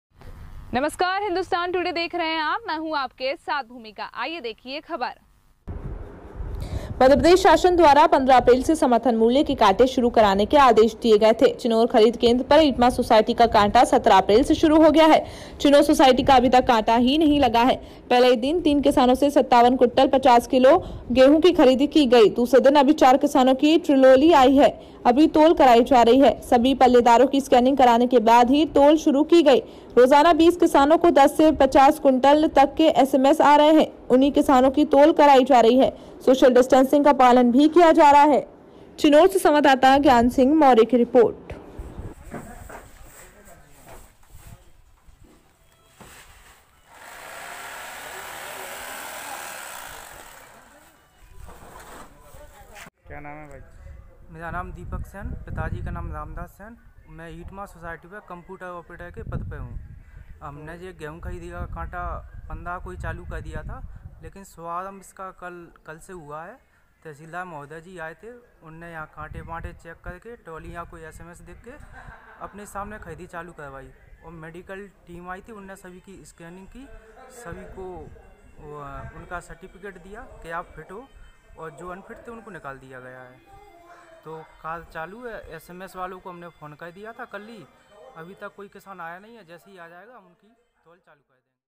म 0 प्र0 शासन द्वारा 15 अप्रैल से समर्थन मूल्य के कांटे शुरू कराने के आदेश दिए गए
म 0 प्र0  शासन द्वारा 15 अप्रैल से समर्थन मूल्य के कांटे शुरू कराने के आदेश दिए गए थे         चीनौर खरीद केंद्र पर ईटमां सोसायटी का कांटा 17 अप्रैल से शुरू हो गया है चीनौर सोसाइटी का अभी तक कांटा ही नहीं लगा है

पहले ही दिन 3 किसानों से 57 कुंटल 50 किलो गेहूं की खरीदी की गई दूसरे दिन अभी 4 किसानों की ट्रिरोली आई हैं उनकी तोल कराई जा रही है

वाइट/कंप्यूटर ऑपरेटर      दीपक सेन/                        सभी पल्लेदारों की स्कैनिंग कराने के बाद ही तोल शुरू की गई रोजाना 20 किसानों को 10 से 50 कुंटल तक के s.m.s. आ रहे हैं उन्हीं किसानों की तौल कराई जा रही है सोशल डिस्टेंसिंग का पालन भी किया जा रहा है अभी कमी ही किसान आ रहे हैं अभी कई किसानों के गेहूं खेतों में खड़े हैं

चीनौर संवाददाता ज्ञानसिंह मौर्य रिपोर्ट

                   की
     इच्छुक युवक-युवतियां
                 या हमें
👇WhatsApp मैसेज करें👇

          inpt न.7067793628